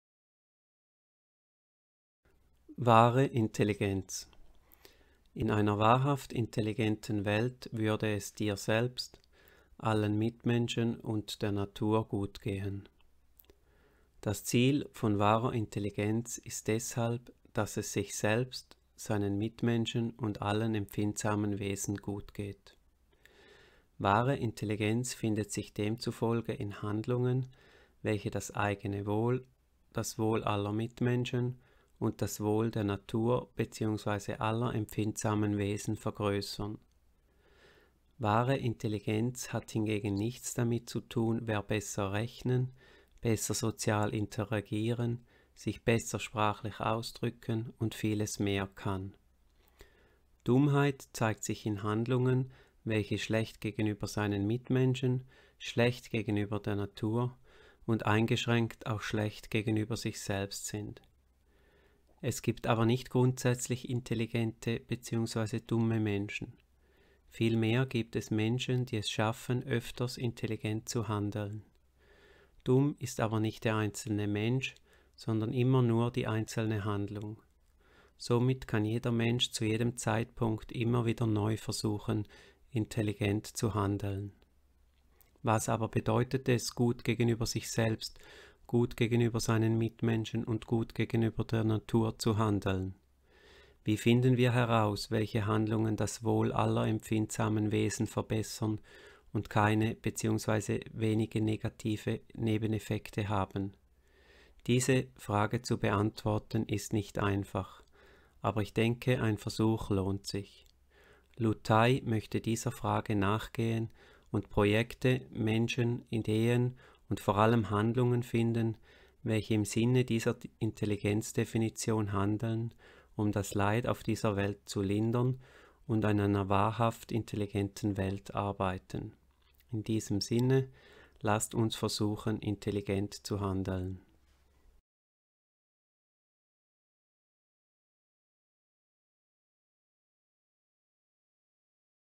Wahre Intelligenz
Was ist wahre Intelligenz?
Handle gut zu dir, allen Mitmenschen und zur Natur.
Der Versuch einer sinnvollen Intelligenzdefinition.
Intelligenz auf die es ankommt.